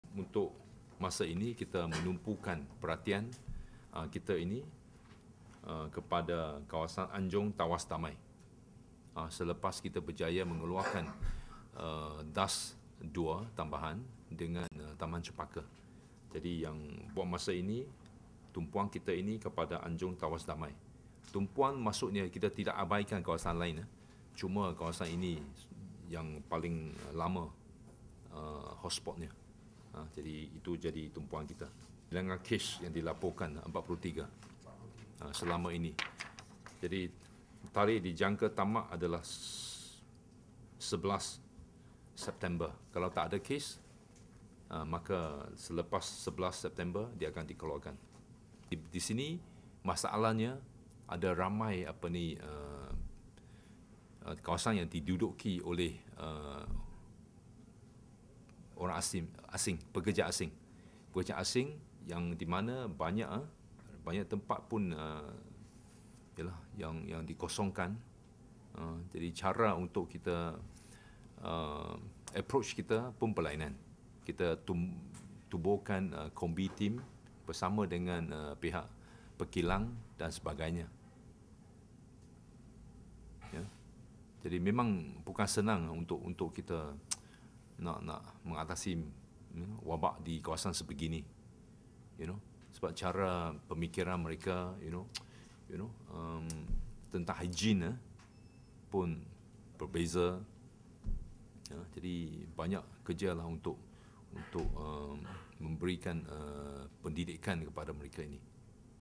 Perbezaan Pemikiran, Cabaran Tangani Denggi Di Anjung Tawas Damai:- Datuk Dr Mah Hang Soon
09/09/2015 -- Perbezaan Pemikiran, Cabaran Tangani Denggi Di Anjung Tawas Damai:- Datuk Dr Mah Hang Soon

Sasaran kerajaan negeri adalah untuk mengeluarkan kawasan Anjung Tawas Damai daripada senarai kawasan panas atau ‘hotspot’ denggi di negara ini.

Menurut Pengerusi Jawatankuasa Kesihatan, Datuk Dr Mah Hang Soon berkata cabaran yag dihadapi pihaknya adalah untuk berhadapan dengan masyarakat di kawasan berkenaan yang rata-ratanya pekerja asing.

“Di sini masalah ada ramai yang diduduki oleh pekerja asing yang mana banyak tempat (jadi kediaman mereka). Jadi cara dan pendekatan kita juga adalah berlainan.

“Bukan senang hendak atasi wabak di kawasan sebegini kerana cara pemikiran mereka berbeza,” katanya pada sidang media selepas mesyuarat exco di Ipoh hari ini.

Menerusi laporan yang diterima, Datuk Dr Mah berkata pihaknya mendapati sebanyak 43 kes denggi dicatatkan di Anjung Tawas Damai yang mana sekiranya kadar itu tidak menurun sehingga 11 September ini, maka kawasan itu terus berada dalam senarai hotspot.

Sebelum ini kerajaan negeri Berjaya mengeluarkan kawasan Kg Dato Ahmad Said Tambahan 2 dan Taman Cempaka daripada 10 senarai hotspot denggi di Malaysia.

Dalam perkembangan yang sama, Datuk Dr Mah memberitahu trend kes denggi di Perak sehingga minggu ke-35 mencatatkan penurunan.

Bagaimanapun dari segi perbandingan jumlah kes pada tahun 2014, kes denggi tahun ini adalah meningkat.

“Sehingga 5 September, kes denggi kumulatif ialah sebanyak 7,181 kes meningkat 92 peratus berbanding tempoh yang sama tahun 2014 iaitu meningkat 3,741 kes.

“Dari segi kematian pula, seramai 22 orang iaitu meningkat 92 peratus,” jelas beliau lagi.

Selain Anjung Tawas Damai, kerajaan negeri sentiasa memberikan penekanan tangani denggi di seluruh kawasan.

“Apa yang penting kita jaga alam persekitaran, itu yang paling penting,” nasihat Datuk Dr Mah.